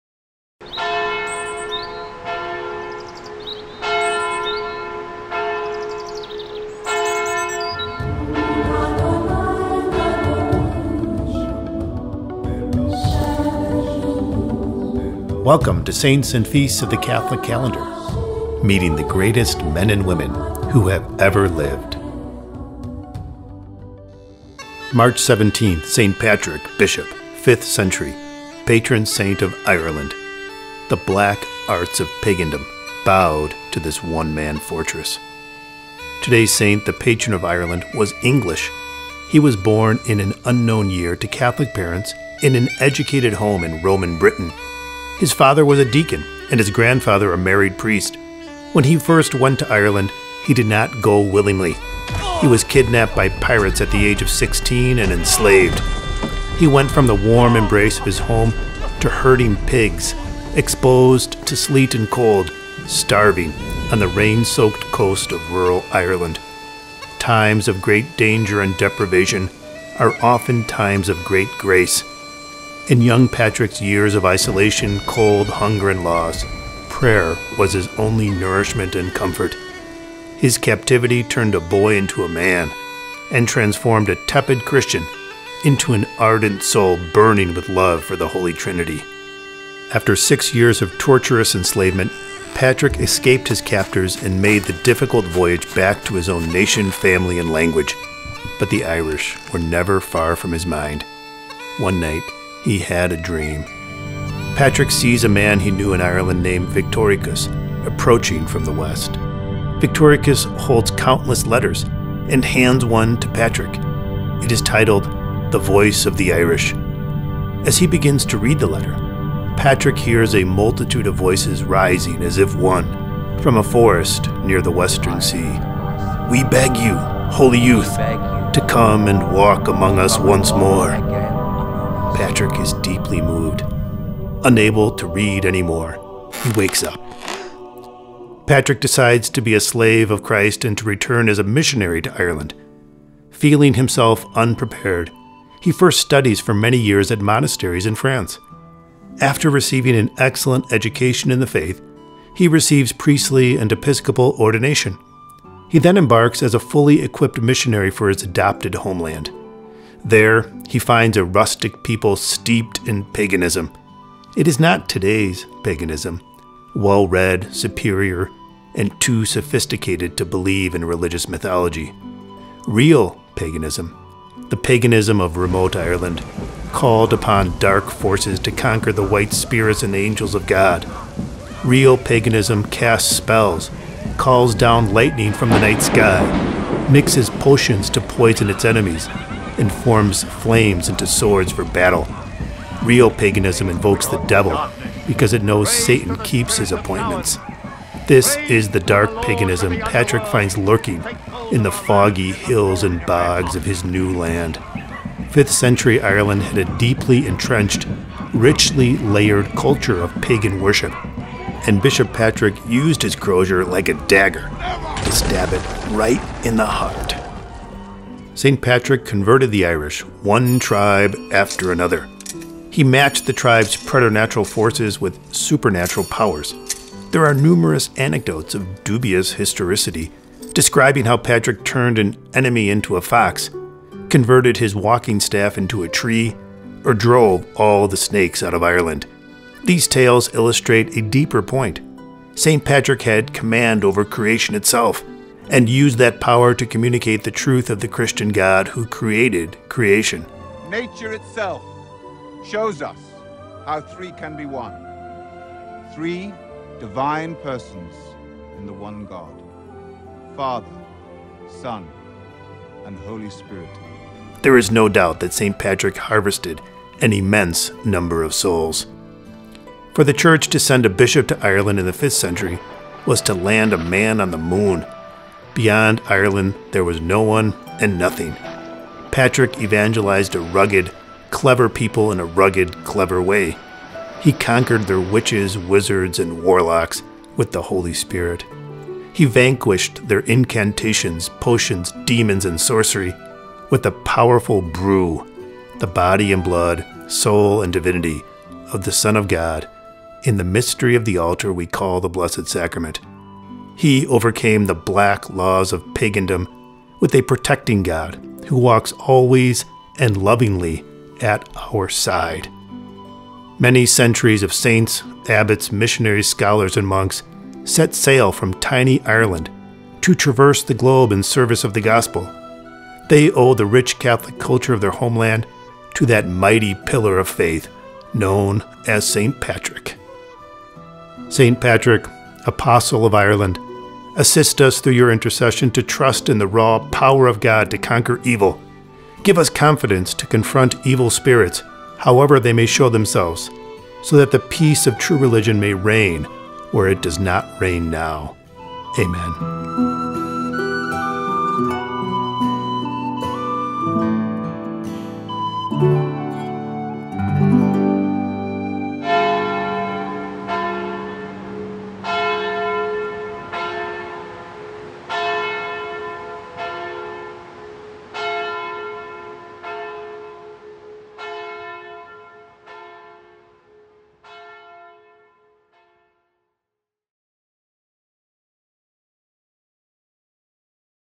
March 17: St. Patrick
The black arts of pagandom bowed to this one-man fortress

Today’s saint, the Patron of Ireland, was English. He was born in an unknown year to Catholic parents in an educated home in Roman Britain. His father was a deacon and his grandfather a married priest. When he first went to Ireland, he did not go willingly. He was kidnapped by pirates at the age of sixteen and enslaved. He went from the warm embrace of his home to herding pigs, exposed to sleet and cold, starving on the rain-soaked coast of rural Ireland. Times of great danger and deprivation are often times of great grace. In young Patrick’s years of isolation, cold, hunger, and loss, prayer was his only nourishment and comfort. His captivity turned a boy into a man and transformed a tepid Christian into an ardent soul burning with love for the Holy Trinity.

After six years of torturous enslavement, Patrick escaped his captors and made the difficult voyage back to his own nation, family, and language. But the Irish were never far from his mind. One night, he had a dream. Patrick sees a man.